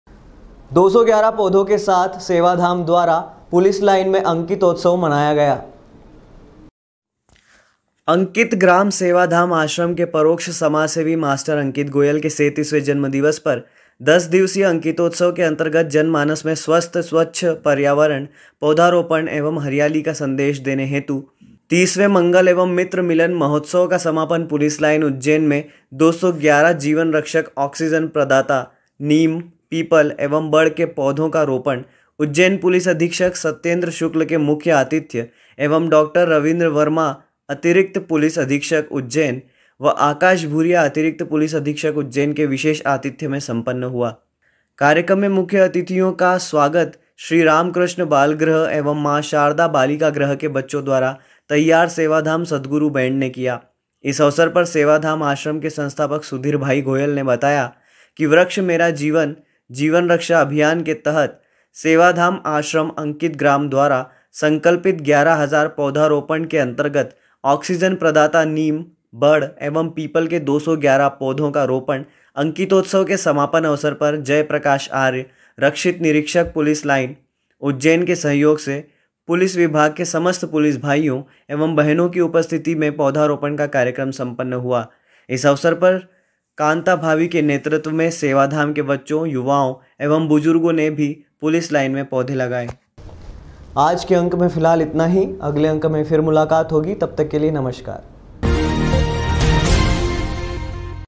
सेवा धाम परिवार द्वारा मनाया अंकितोत्सव, पुलिस लाइन मैं लगाए 211 पौधे
अंकित ग्राम’ सेवाधाम आश्रम के परोक्ष समाज सेवी मास्टर अंकित गोयल के  37वें जन्मदिवस पर 10 दिवसीय अंकितोत्सव के अंतर्गत जनमानस में स्वस्थ्य-स्वच्छ पर्यावरण, पौधारोपण एवं हरियाली का संदेश देने हेतु 30वें वर्षा मंगल एवं मित्र मिलन महोत्सव का समापन पुलिस लाईन उज्जैन में 211 जीवन रक्षक आक्सीजन प्रदाता नीम, पीपल एवं बड़ के पौधों का रोपण उज्जैन पुलिस अधीक्षक सत्येन्द्र शुक्ल के मुख्य आतिथ्य एवं डाॅ. रविन्द वर्मा अतिरिक्त पुलिस अधीक्षक उज्जैन व आकाश भूरिया अतिरिक्त पुलिस अधीक्षक उज्जैन के विशेष आतिथ्य में सम्पन्न हुआ।